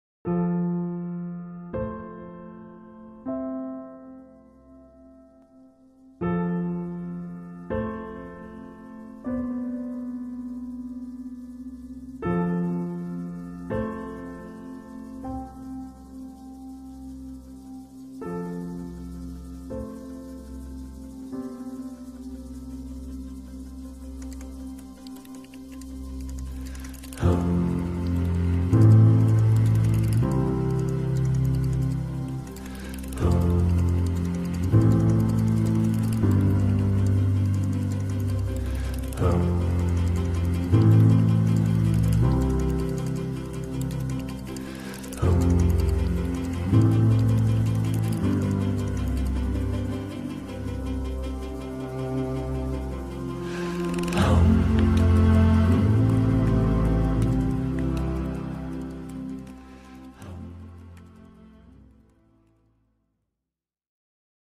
Lusowo z lotu ptaka
Lusowo widziane z lotu ptaka. DJI Phantom 2 gopro3+ .